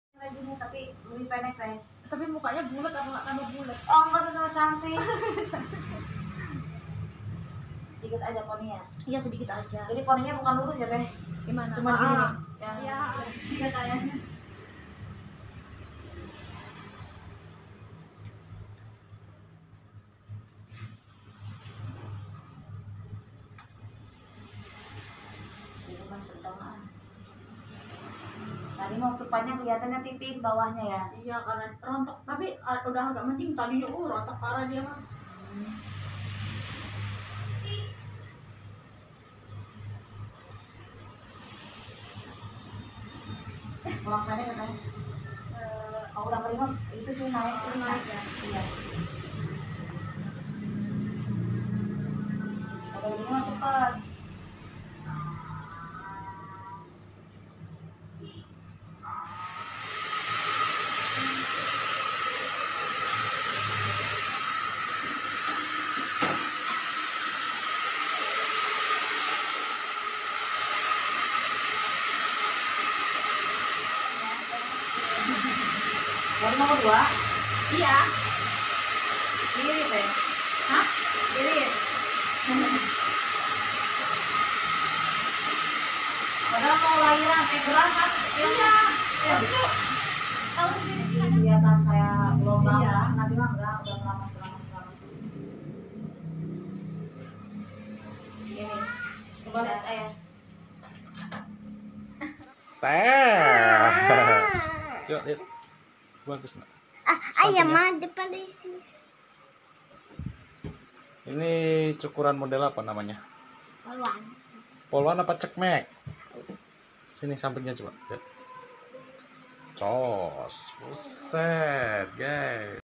Potong Rambut Anak Perempuan Model Bob, Cocok Untuk Rambut Yang Tipis Dan Tebal
New Model Girls Hair Cuts Suitable For Thin And Thick Hair